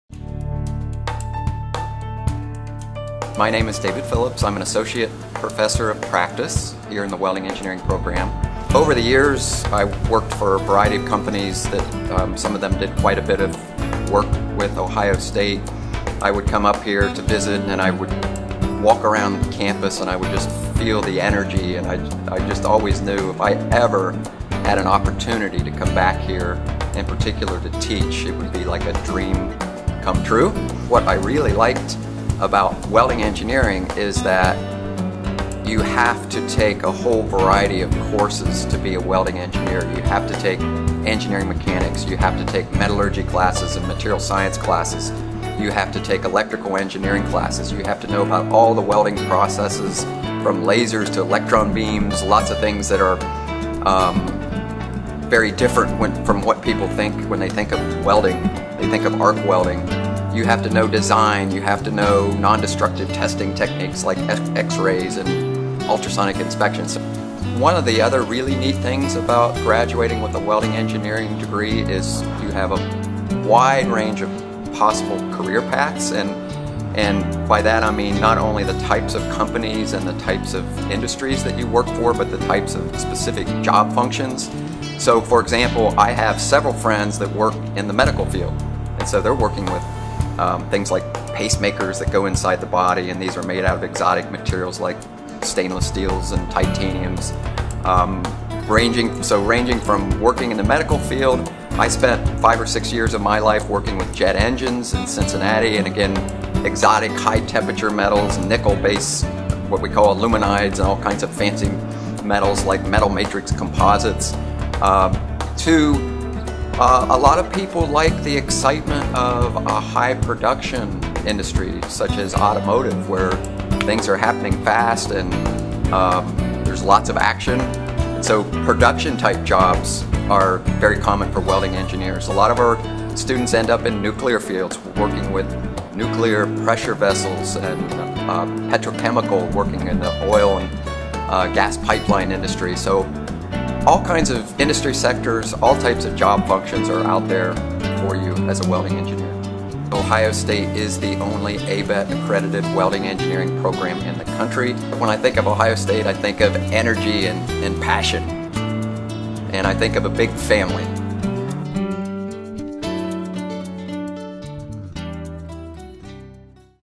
Professor David Phillips on What You Can Do with a Welding Engineering Degree
The Ohio State University Welding Engineering Program would like to invite you to explore what it means to be a Welding Engineering Major.